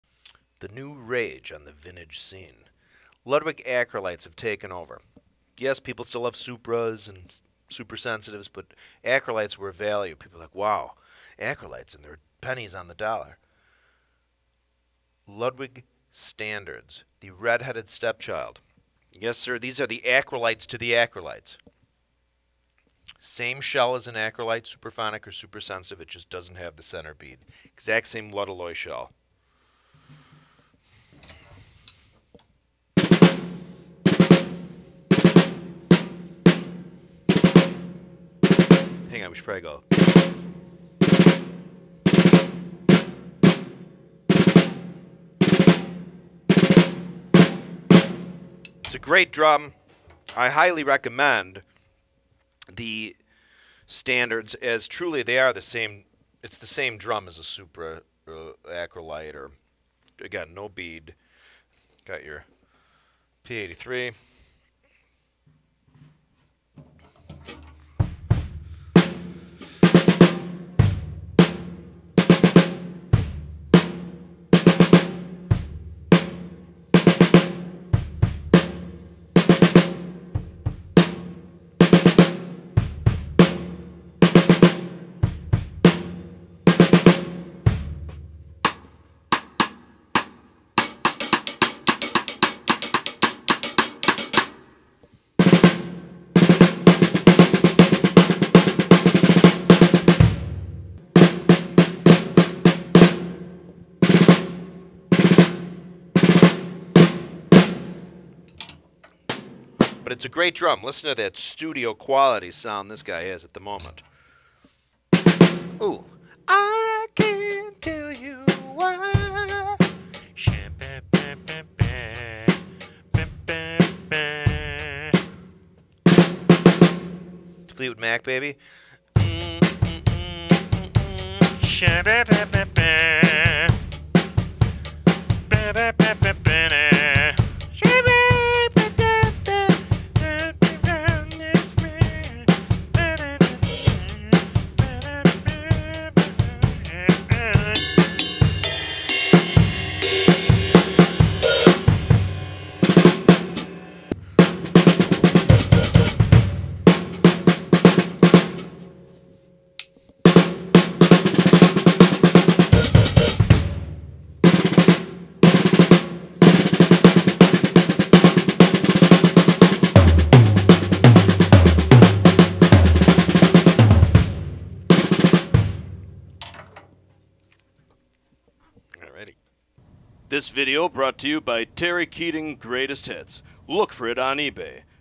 THE MERITS OF THE *LUDWIG STANDARD* SNARE DRUM (LUDALOY-SHELLED)
Yessir! Vintage Ludwig Ludaloy-shelled STANDARD snare drums!
Get one NOW! They're about to skyrocket in popularity!